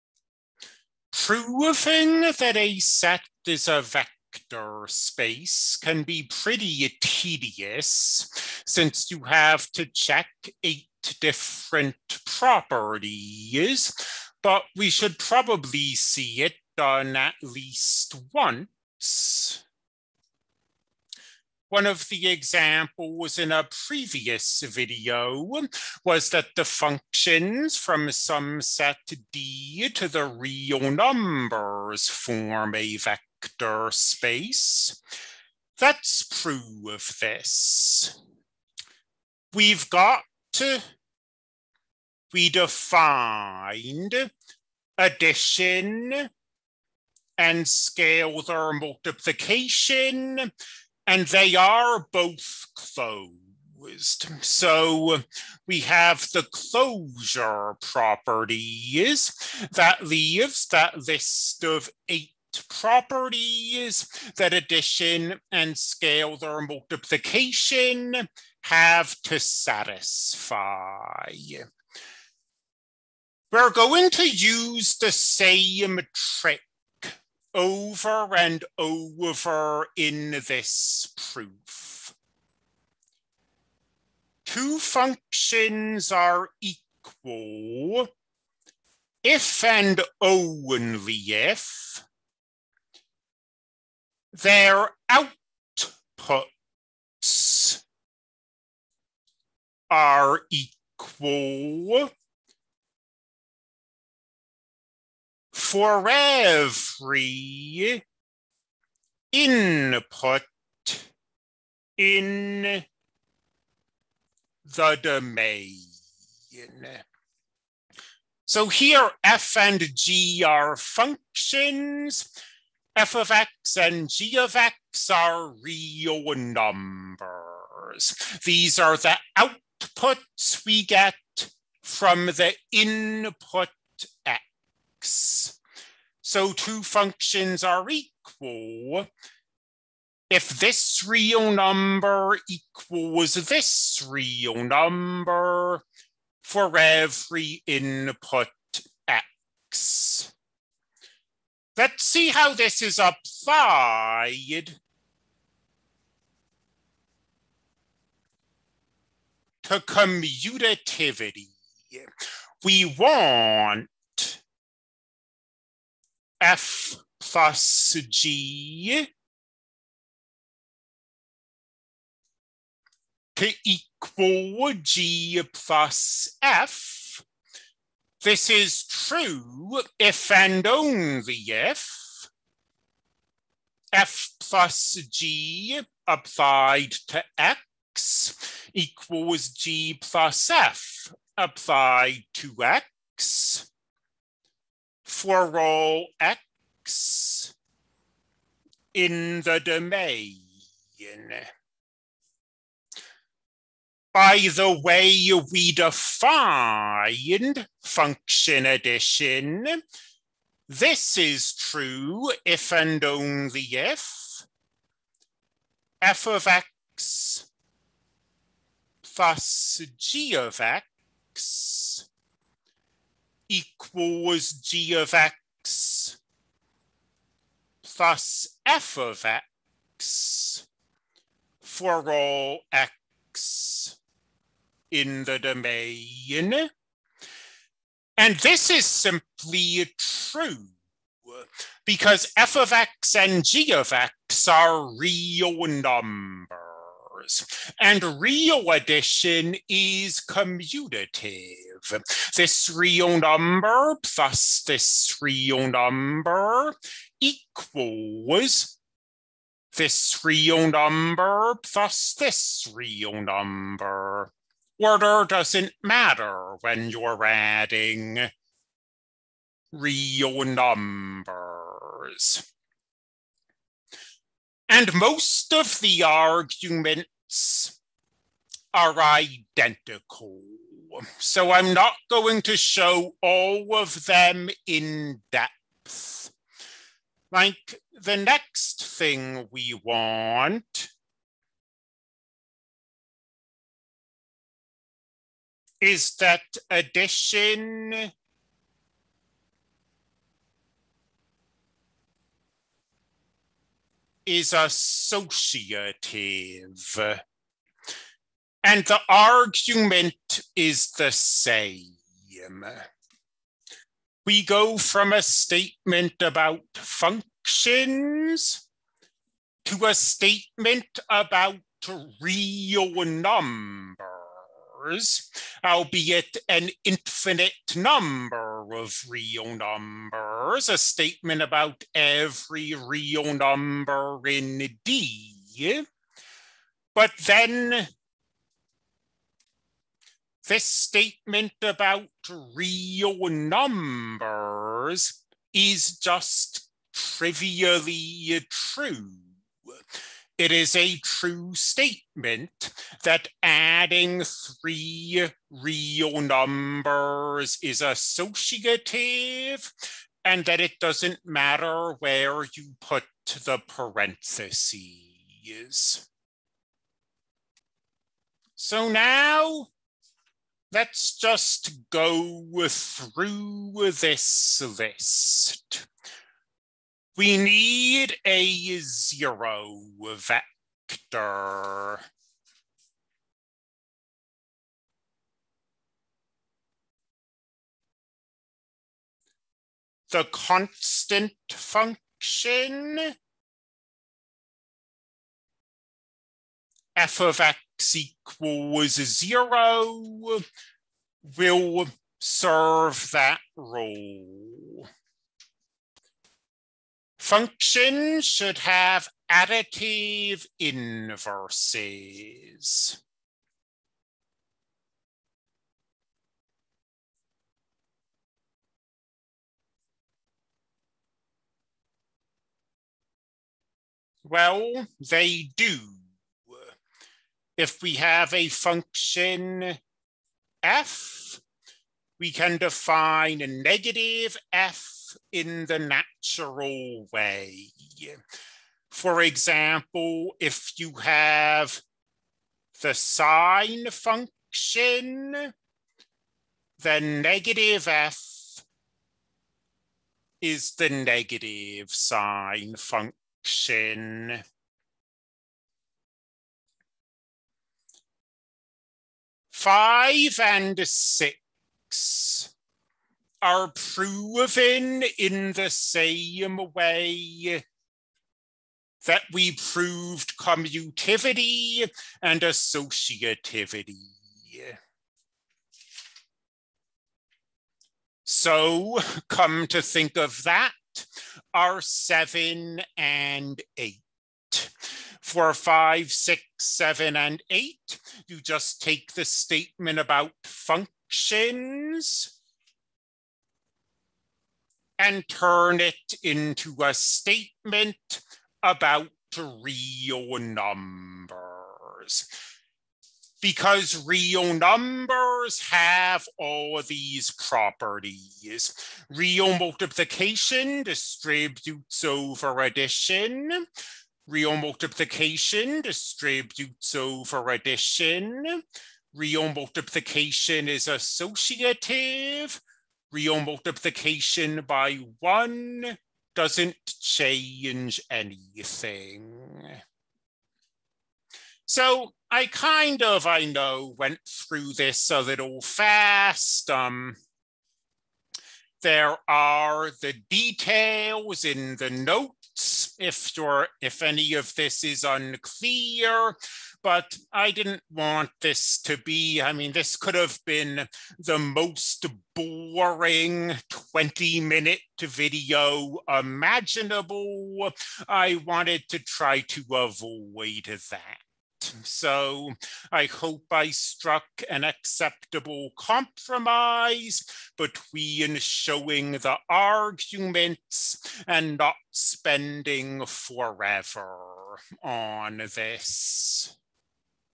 LA, Section 4 1, Vector space of functions
The set of real-valued functions with some common domain D is a vector space.  This material comes from Section 4.1 of our textbook.